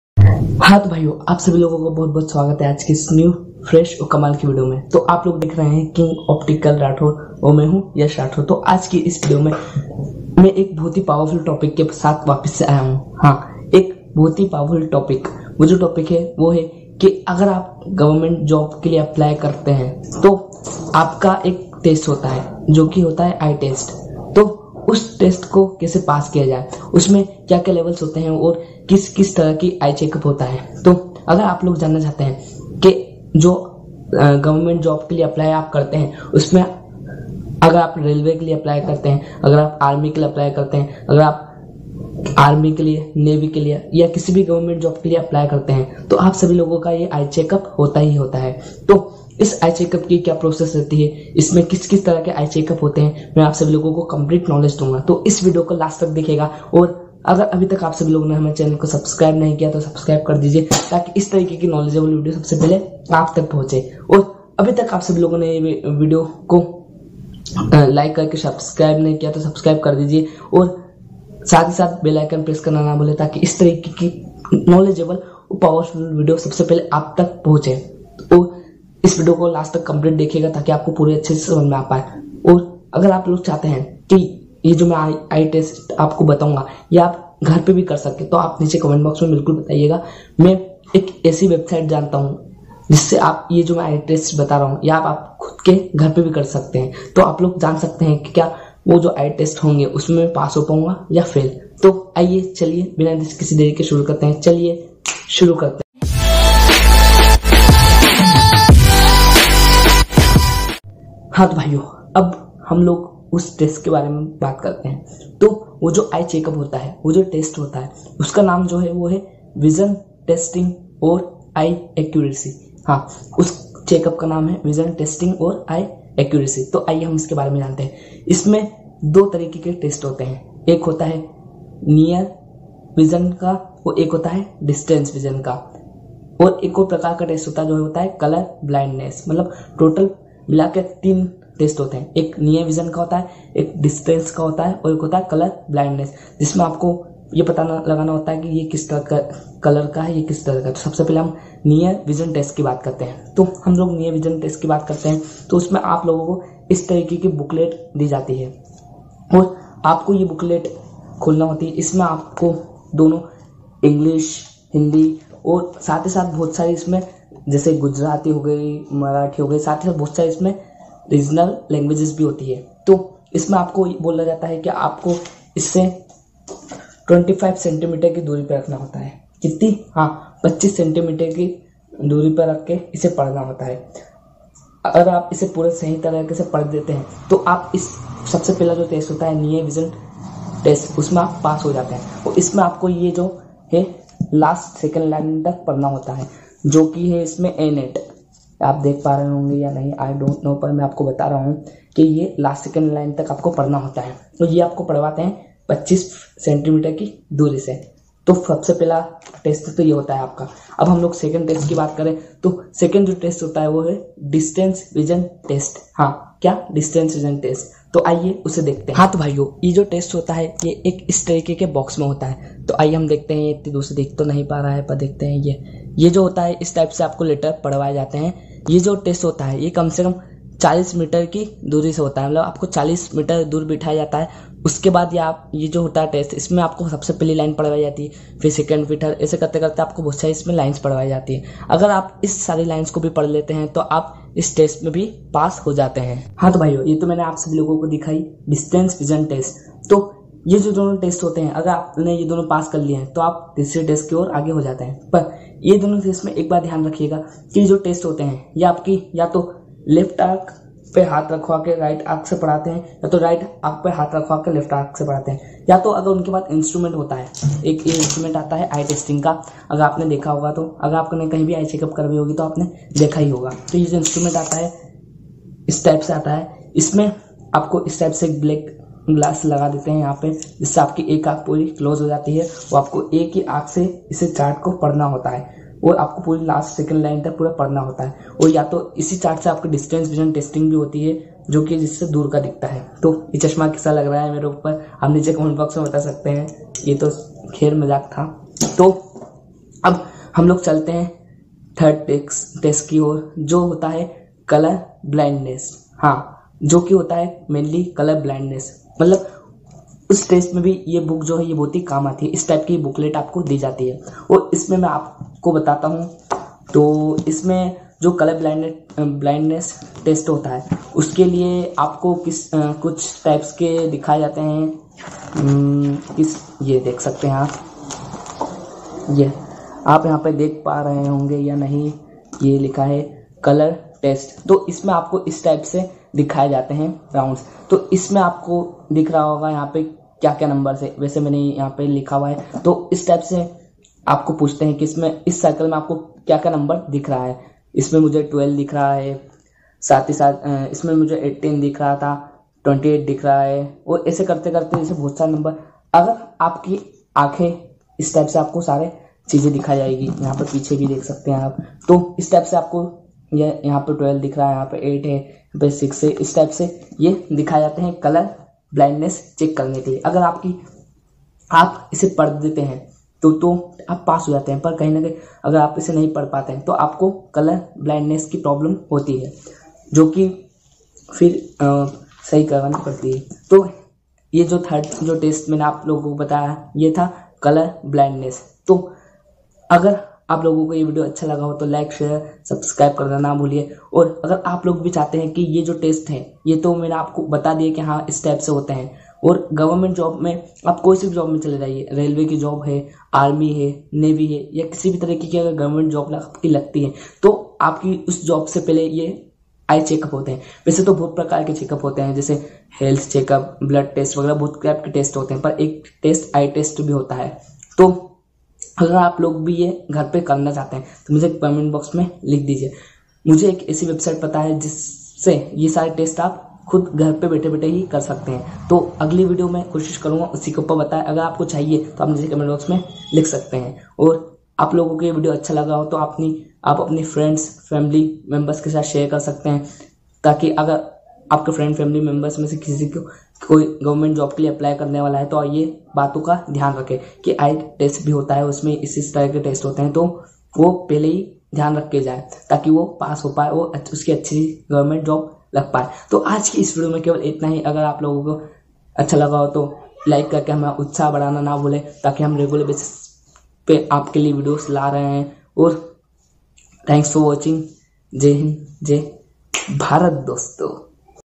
100% pass eye medical test in government job/eye-testing for govt.Job/eye-checkup
hello dosto isvideo mene apko complete knowledge Diya hai government eye exam examination ke bare mein. to please complete is video ko dekhiae agar aap government job chahte Hain to.